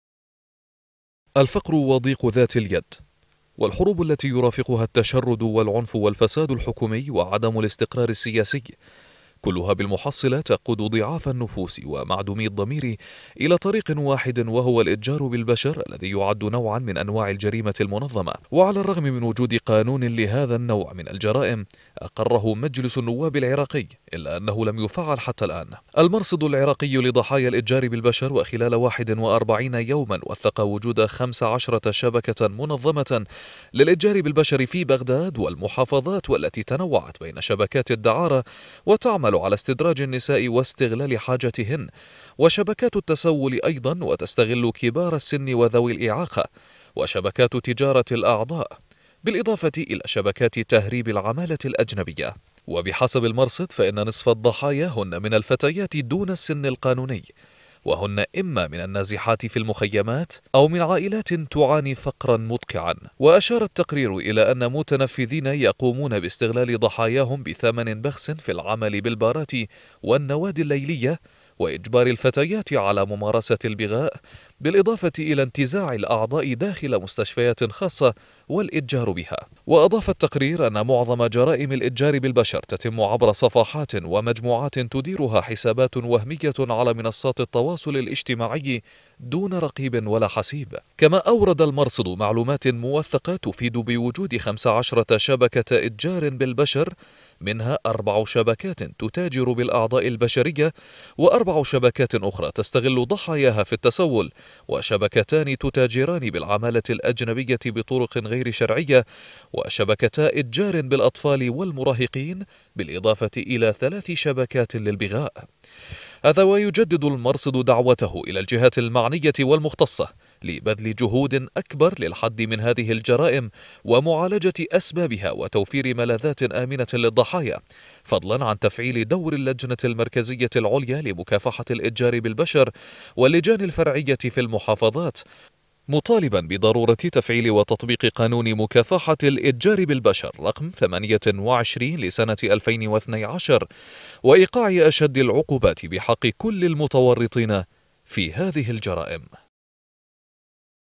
تقرير المرصد العراقي لضحايا الإتجار بالبشر
من احدى الظواهر السلبية الخطيرة التي انتشرت وزادت رقعة اتساعها في المجتمع العراقي ما بعد الفين وثلاثة، ظاهرة الاتجار بالبشر واستغلال النساء والفتيات والاطفال لأعمال السمسرة والدعارة والتسول.

 تقرير: سكينة محمد سعيد       قراءة: محمد عمر